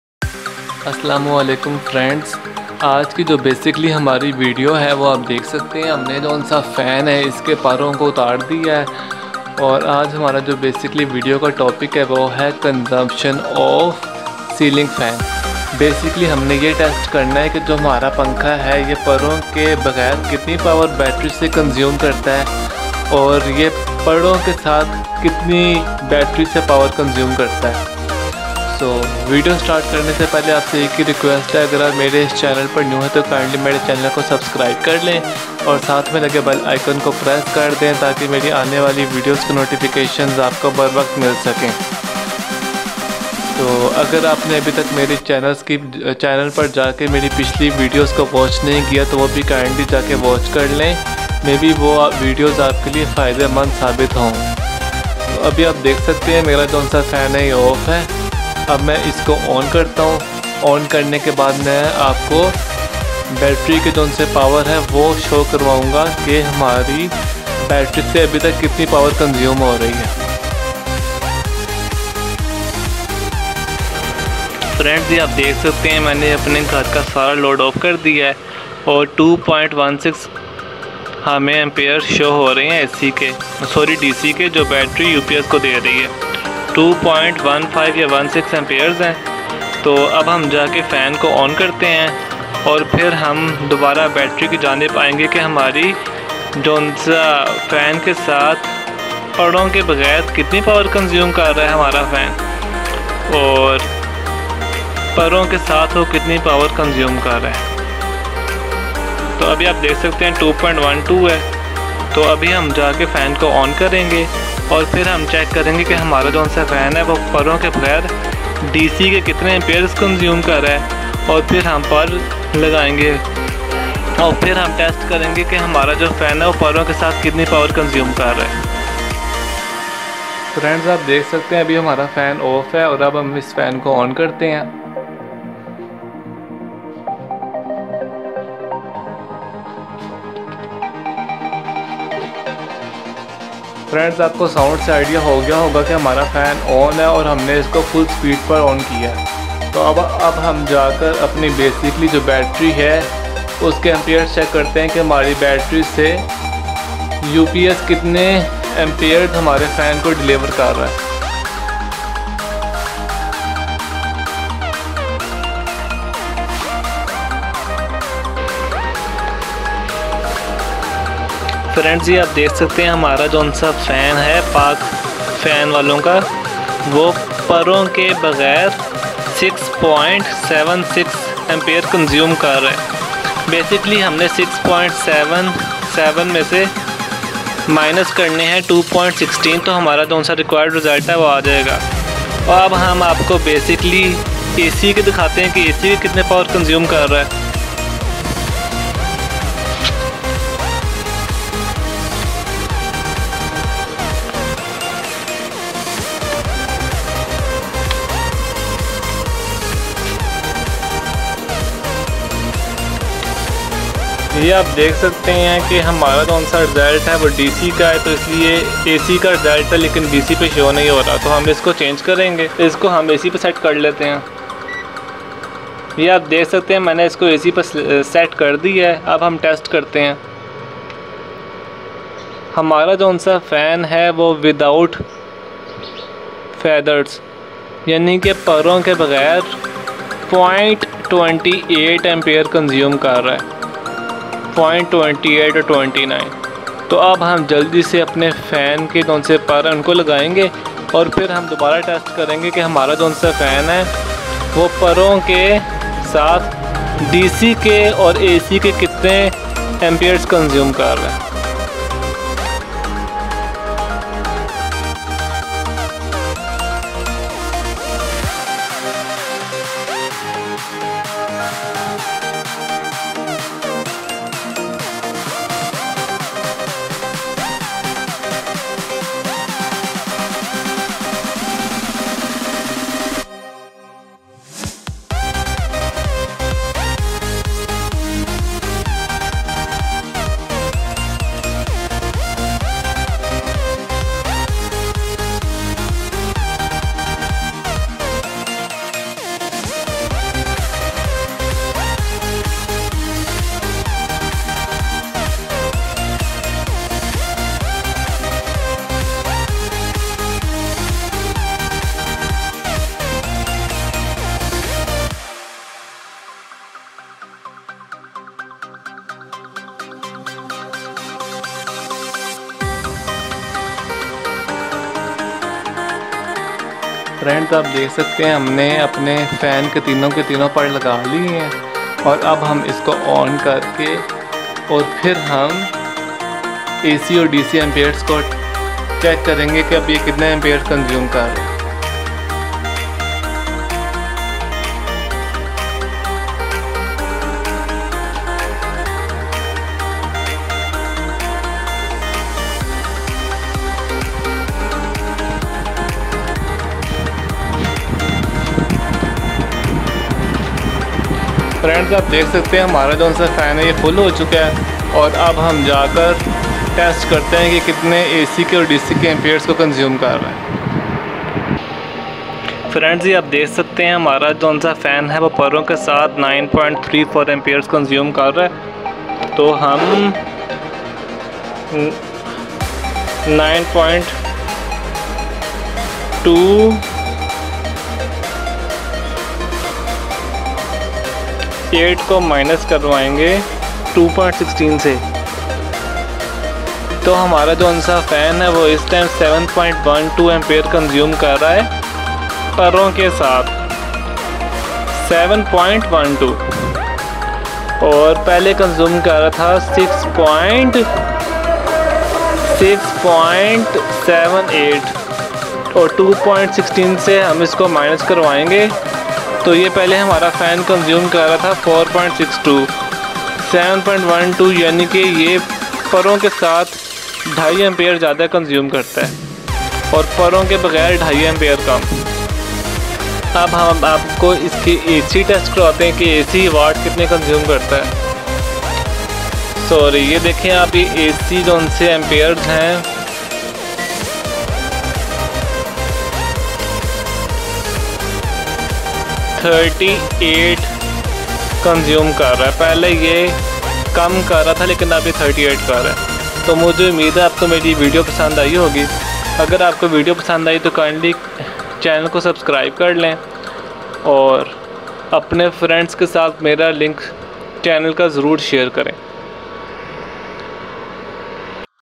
PAK FAN Power Consumption
Power Consumption of PAK FAN is completely explained in this video.